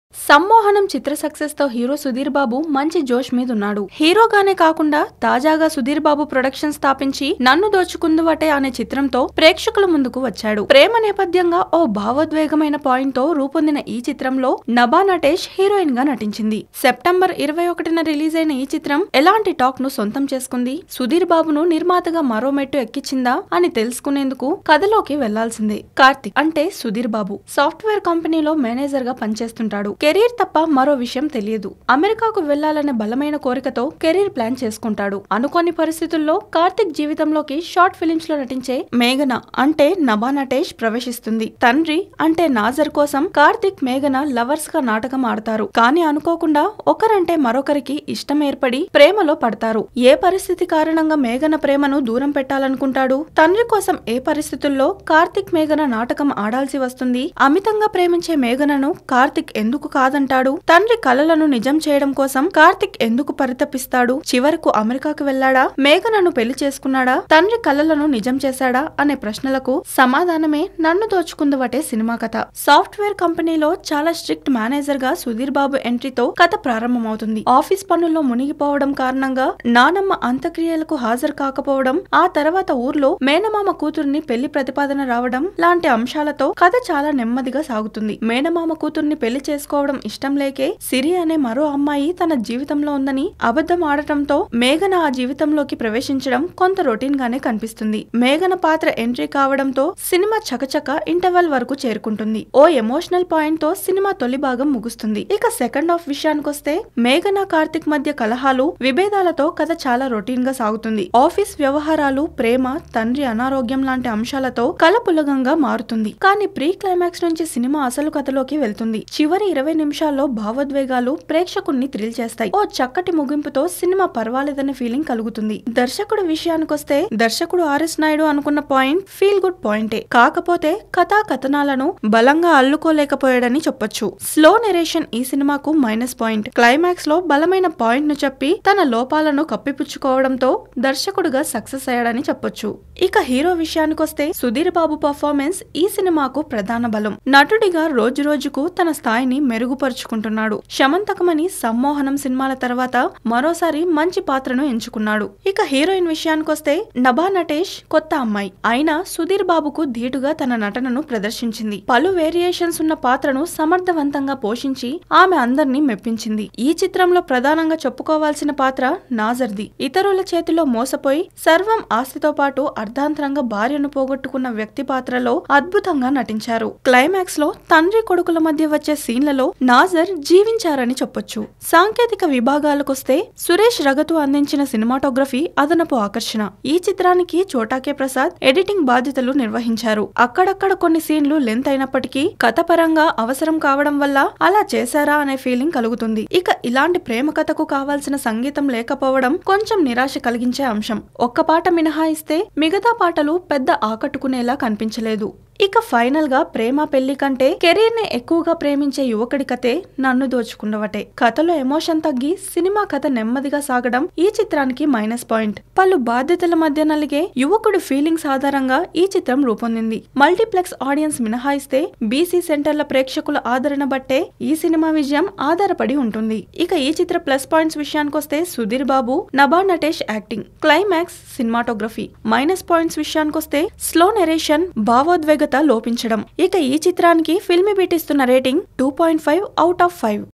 Nannu Dochukunduvate Movie Review | Filmibeat Telugu
Nannu Dochukunduvate revolves around a software company who strictly does his duties, and is a happy go lucky employee. Sudheer Babu Posani, Nabha Natesh lead pairs in this movie. This movie made under sudheer Babu productions. This movie released on September 21st. In this occassion, Telugu  brings exclusive review.

సమ్మోహనం చిత్ర సక్సెస్‌తో హీరో సుధీర్ బాబు మంచి జోష్ మీద ఉన్నాడు. హీరోగానే కాకుండా తాజాగా సుధీర్‌బాబు ప్రొడక్షన్ స్థాపించి నన్ను దోచుకుందువటే అనే చిత్రంతో ప్రేక్షకుల ముందుకు వచ్చాడు. ప్రేమ నేపథ్యంగా ఓ భావోద్వేగమైన పాయింట్‌తో రూపొందిన ఈ చిత్రంలో నభా నటేష్ హీరోయిన్‌గా నటించింది. సెప్టెంబర్ 21 రిలీజైన ఈ చిత్రం ఎలాంటి టాక్‌ను సొంతం చేసుకొంది? సుధీర్ బాబును నిర్మాతగా మరో మెట్టు ఎక్కించిందా? అని తెలుసుకొనేందుకు కథలోకి వెళ్లాల్సిందే.